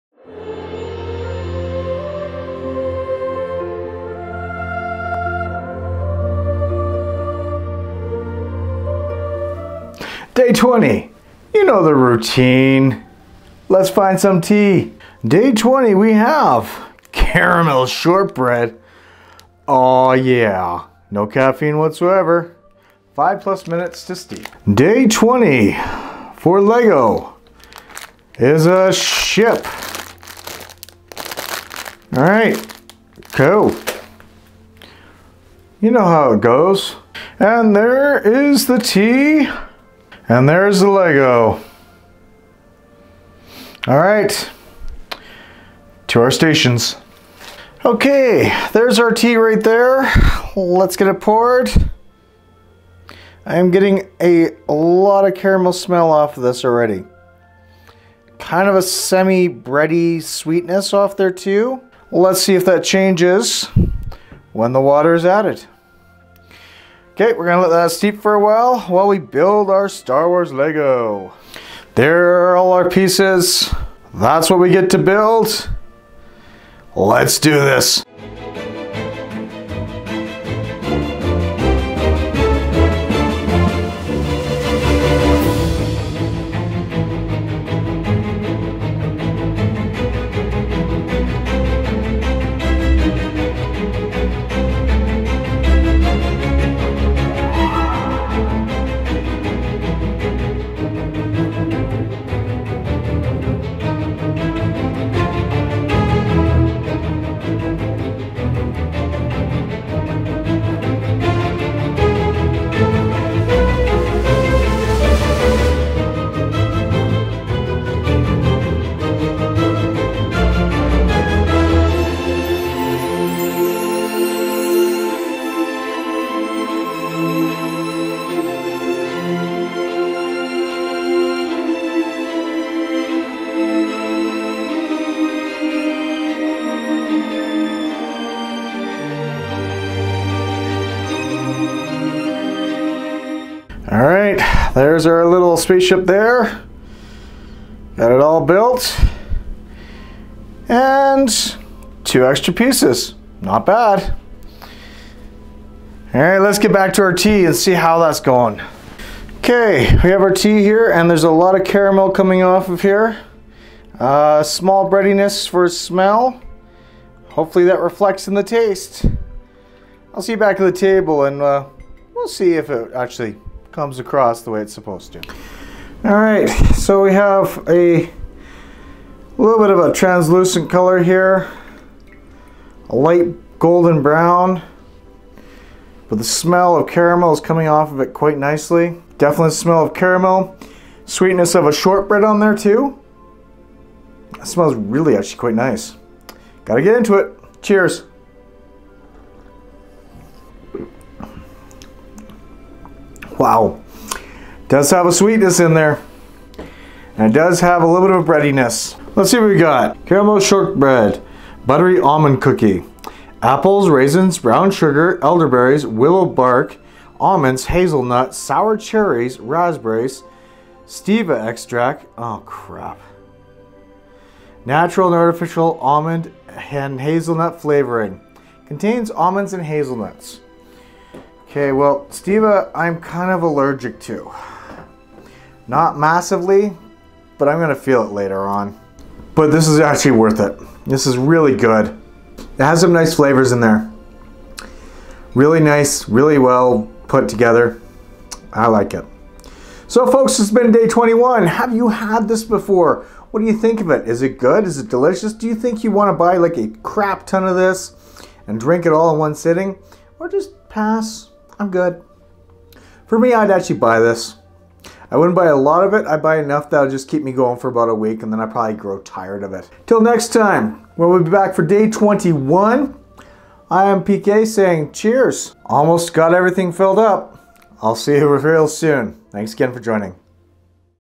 24 Dry Days of December - Day 20 - Caramel Shortbread and another ship
THIS tea is wow.. just wow! I can say nothing else but wow!

The lego was a tough one to build too!